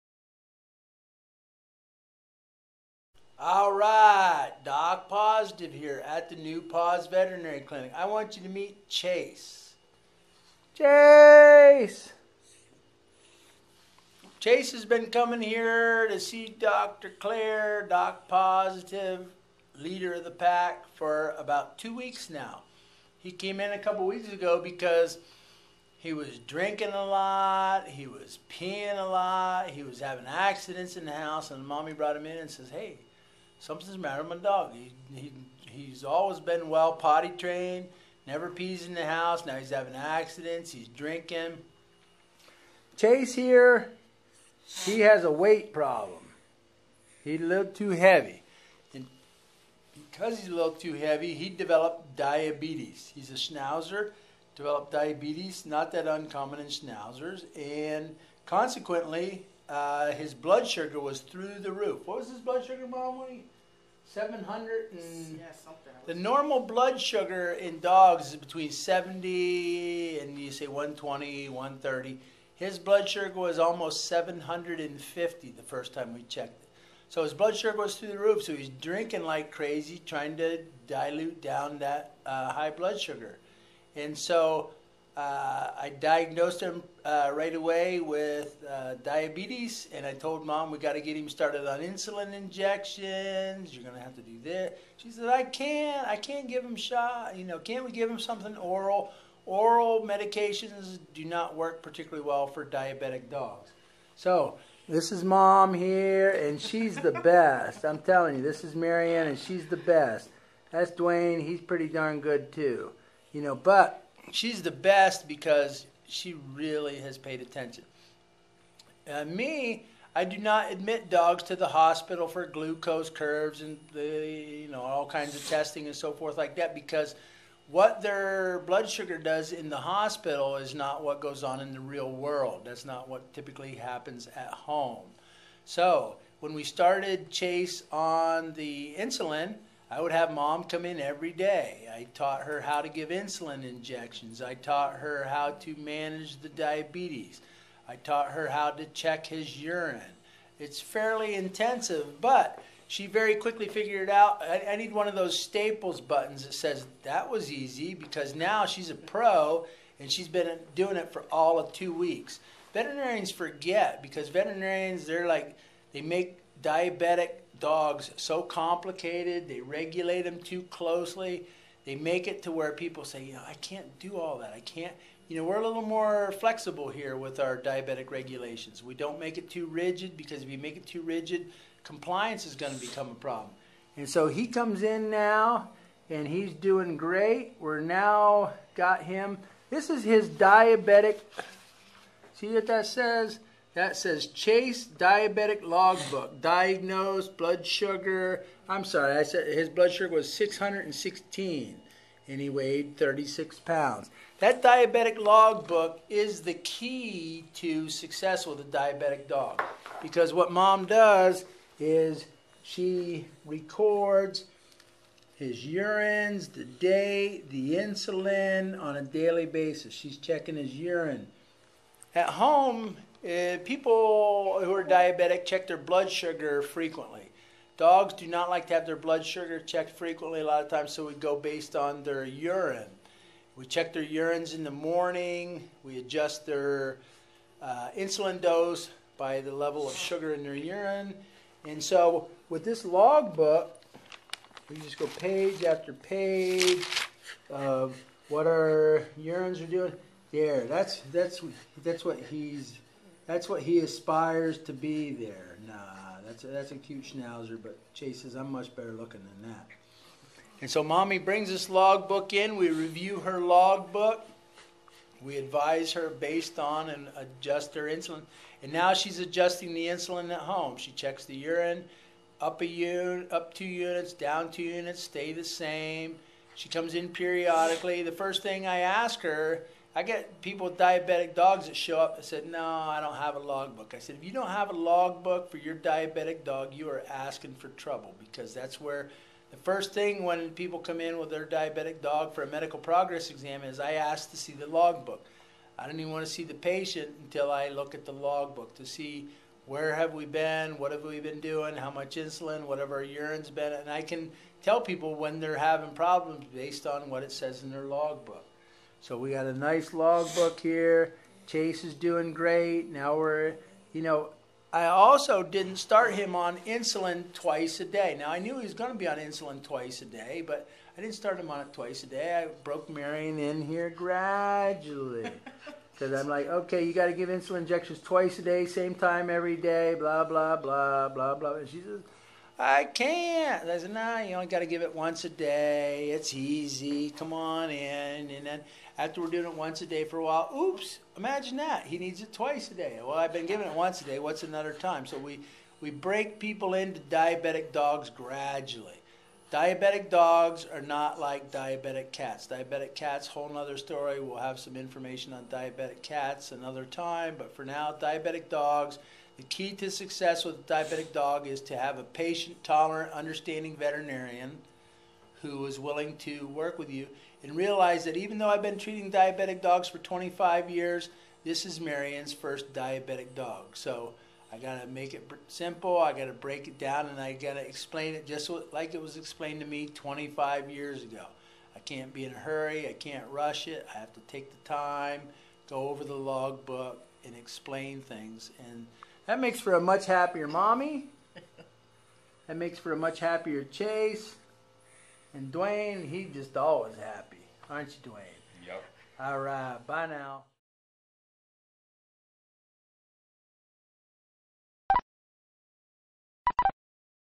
Dog Diabetes Basics
Diabetes mellitus, often simple called diabetes, is a common veterinary problem diagnosed in middle-aged and older overweight dogs. Obesity in dogs increases the risk of the onset of diabetes, same as in humans. Diabetes in dogs is best controlled with daily insulin injections and is relatively simple to control in the majority of diabetic dogs. Owners regulate daily insulin doses either by monitoring blood sugar levels with a home glucose monitoring kit, such as an Alpha Trak Glucometer; or more commonly by checking urine sugar and ketone levels with a urine Keto-Diastix strip. The majority of owners do very well regulating the diabetes at home provided they have been adequately instructed on the basic principles of diabetes, the proper use of insulin and maintenance of a complete diabetic logbook to record sugar levels, urine levels, insulin dosages, appetite and other parameters. Alaways consult a qualified veterinary professsional if you suspect your pet is having a problem. Doc Pawsitive has successfully educated even the most hesitant owners on proper diabetic management of dogs in a home environment!